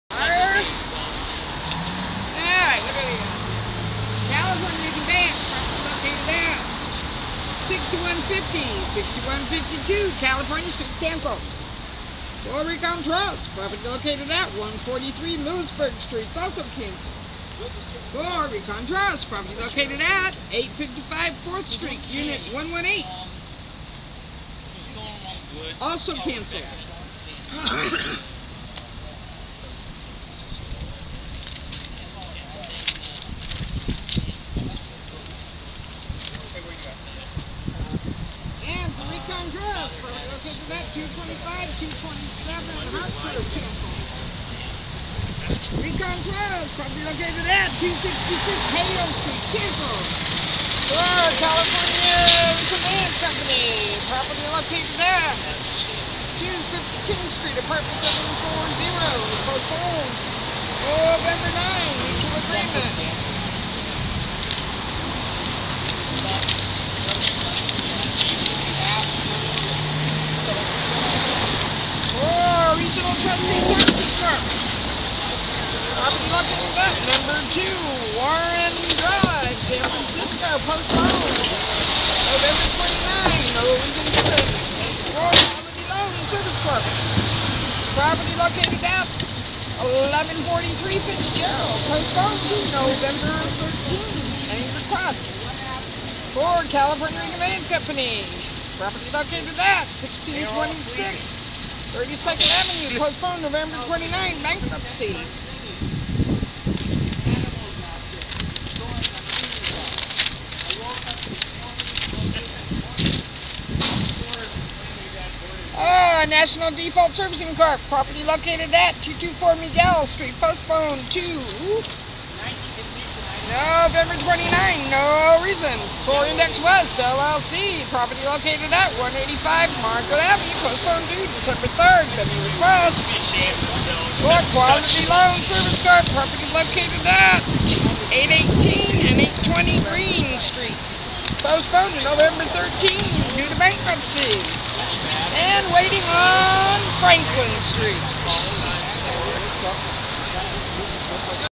Part I of Occupy the Auctions on October 29, 2012, in San Francisco
Part I of Occupy the Auctions on October 29, 2012, in San Francisco (VID00001.MOV)